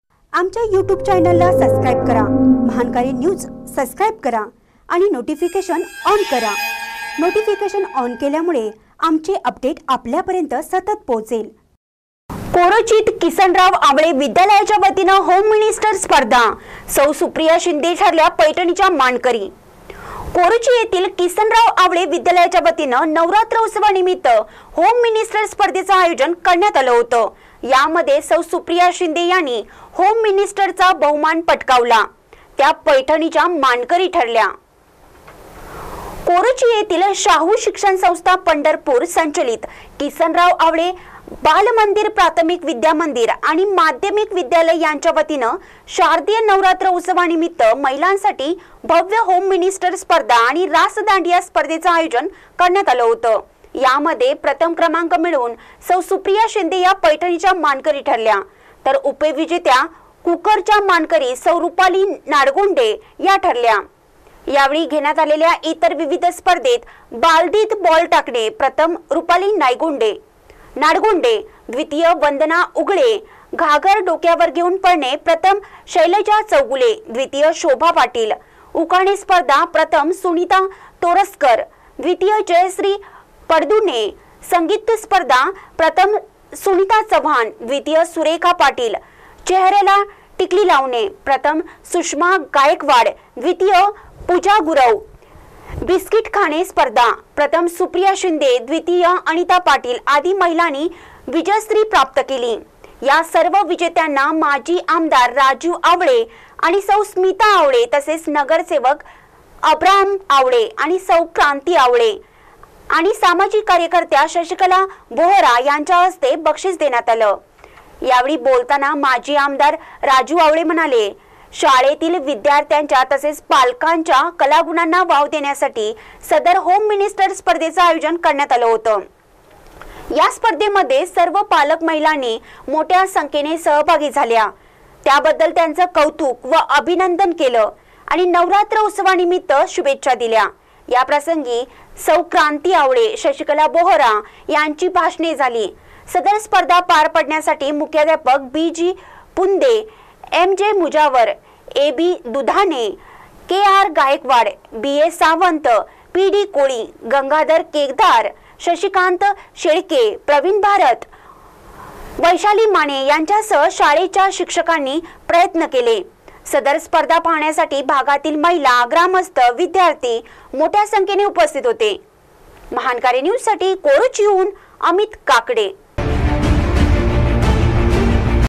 कोरोचीत किसनराव आवळे विद्यालयाच्यावतीने होम मिनिस्टर स्पर्धा :
कोरोचीत किसनराव आवळे विद्यालयाच्यावतीने होम मिनिस्टर स्पर्धा : सौ सुप्रिया शिंदे ठरल्या पैठणीच्या मानकरी